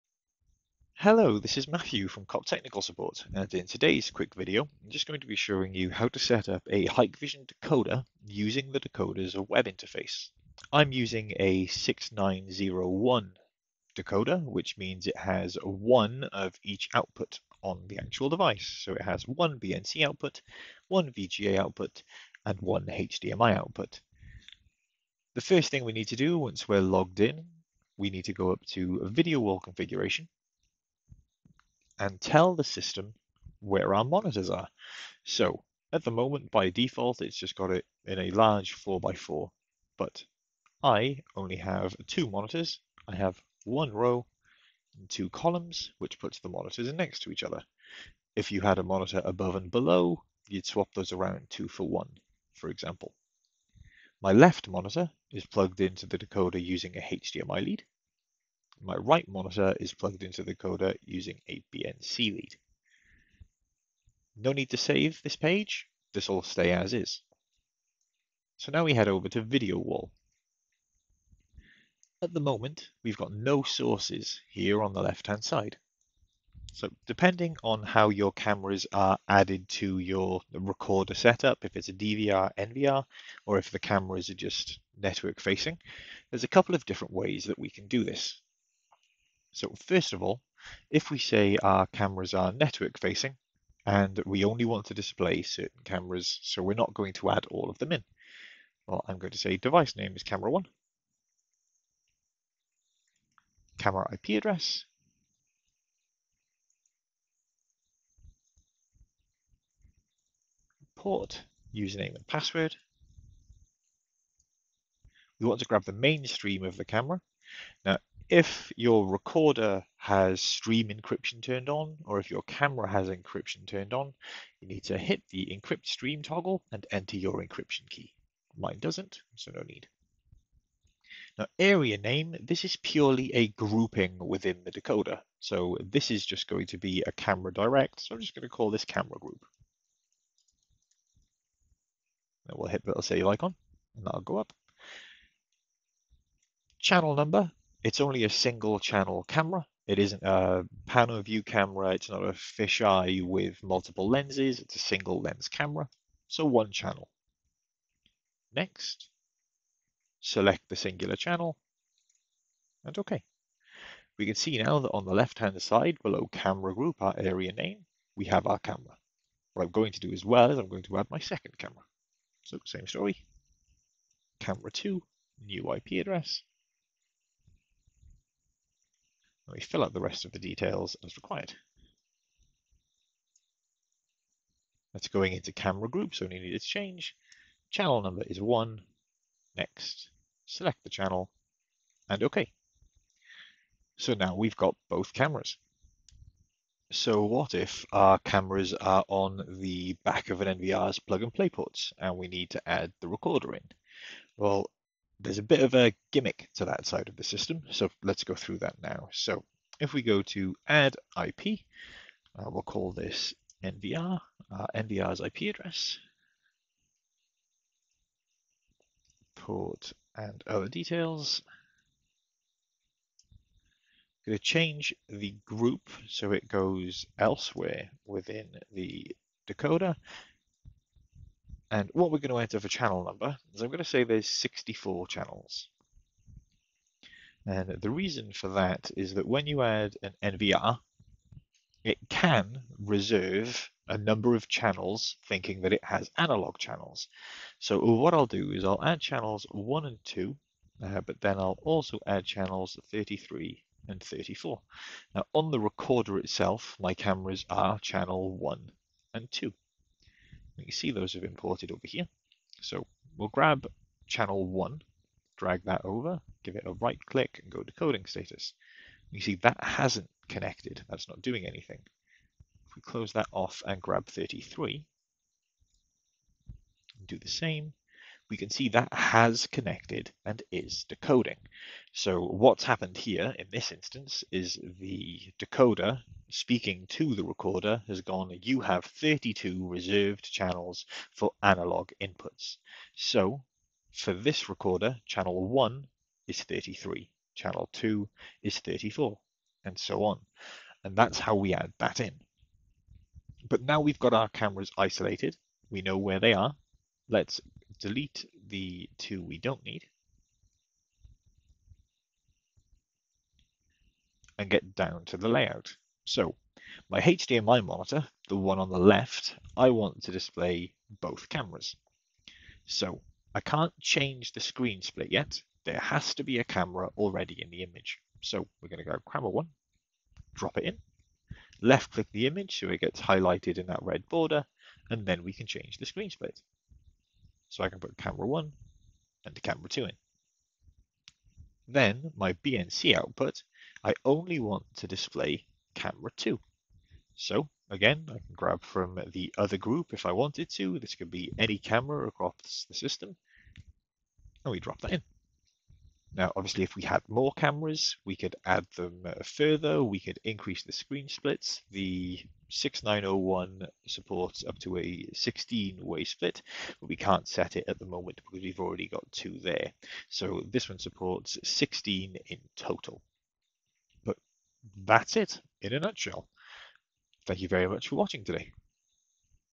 Hikvision Decoder Setup Via Web Interface
Follow along as we show you how to set up a Hikvision decoder through the web UI.

We’ll guide you through the process, ensuring that your smart wall is set up smoothly and efficiently.